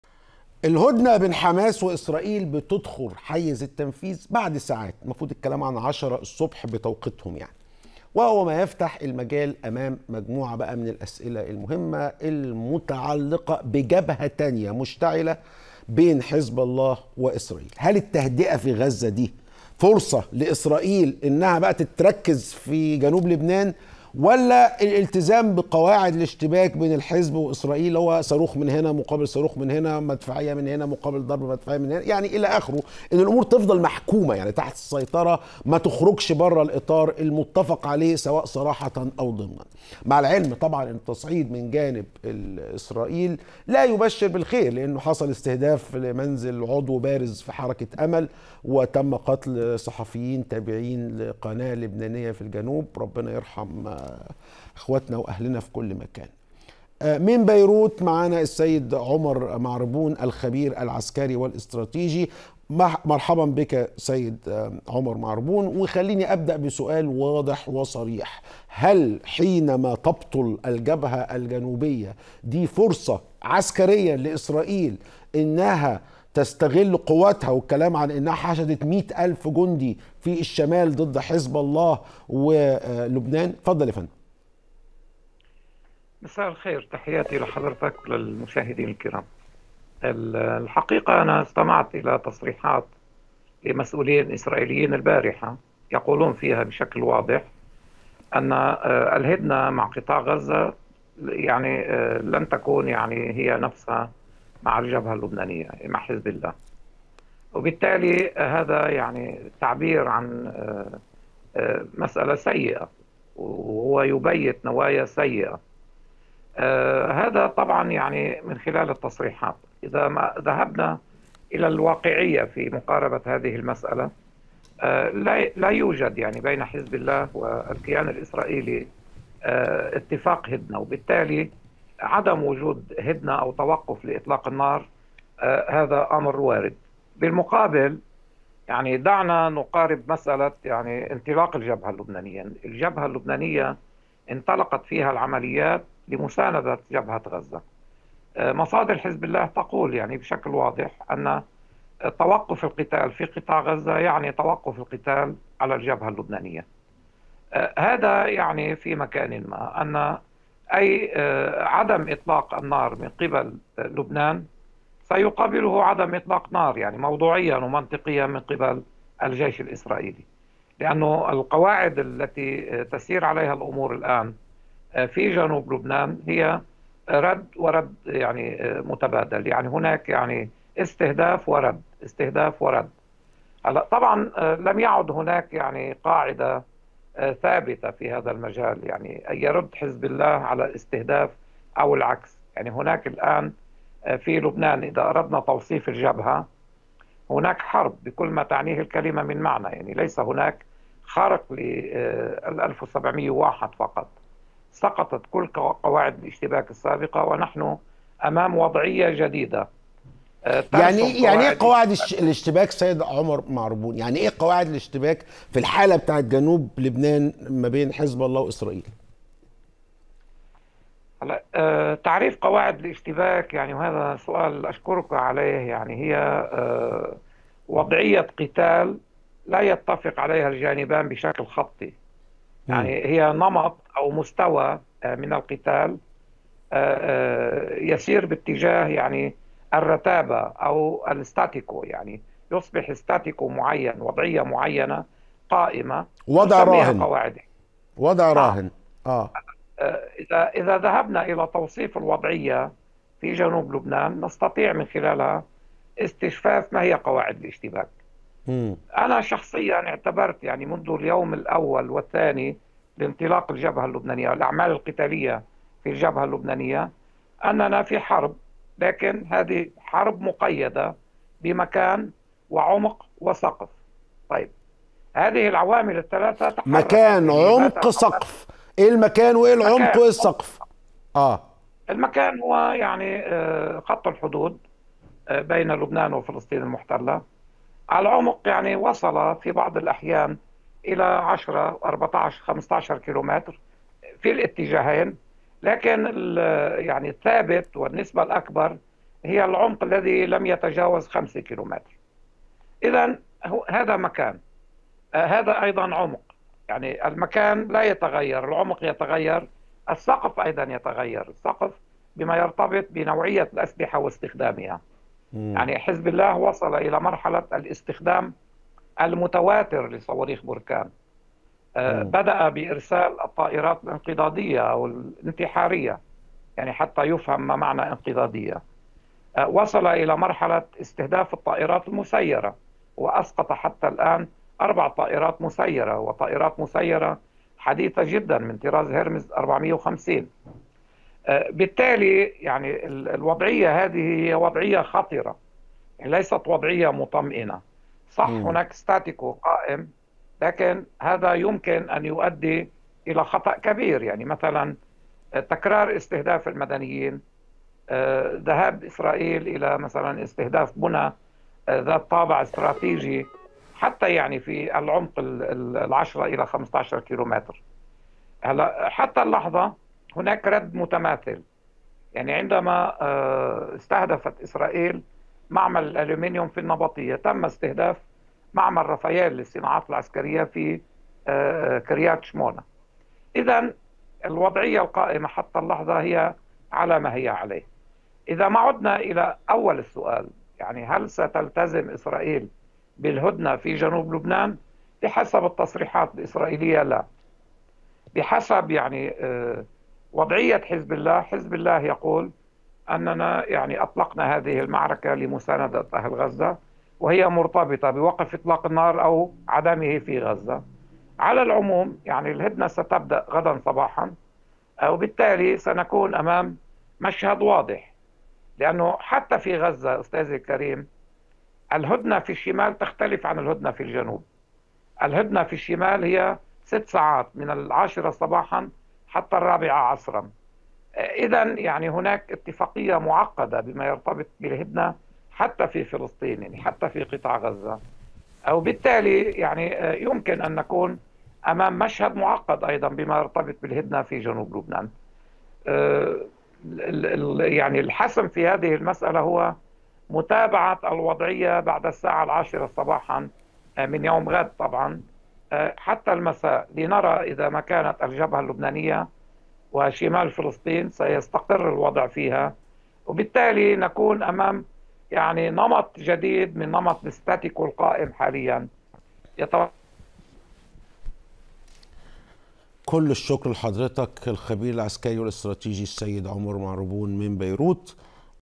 تصريحات ايلي كوهين تشعل الجبهة الشمالية.. هل تتفرغ إسرائيل لـ "حزب الله" بعد إقرار الهدنة؟
الهدنة بين "حماس" وإسرائيل تدخل حيز التنفيذ بعد ساعات، وهو ما يفتح المجال أمام مجموعة من الأسئلة المهمة المتعلقة بجبهة تانية مشتعلة، بين "حزب الله" وإسرائيل،
هل التهدئة في غزة ستشعل جبهة جنوب لبنان؟
تفاصيل أكثر مع الخبير العسكري والاستراتيجي عمر معربون.